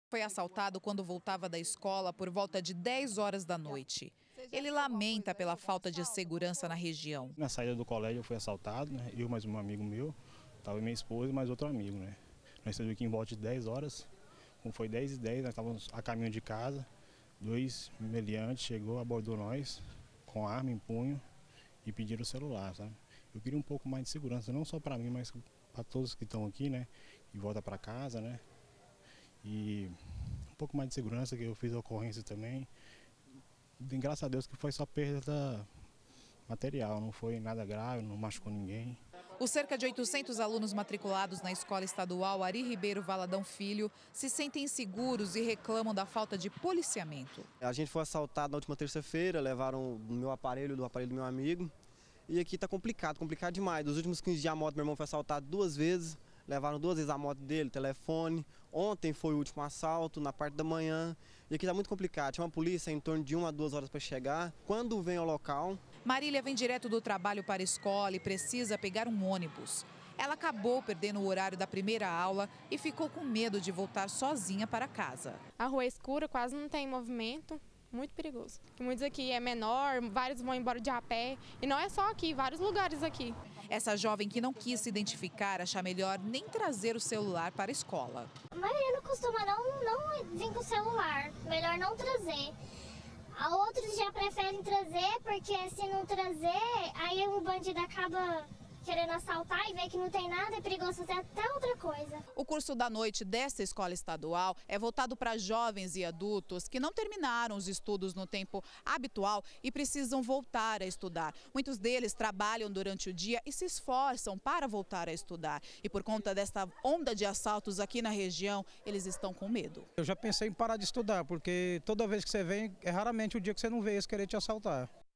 GA - Insegurança: Medo na porta da escola - 10-04-2018
Estudantes de uma escola na Vila Finsocial estão com medo de ir às aulas. Todos os dias têm assalto na porta. Tem gente até pensando em desistir dos estudos por causa da insegurança.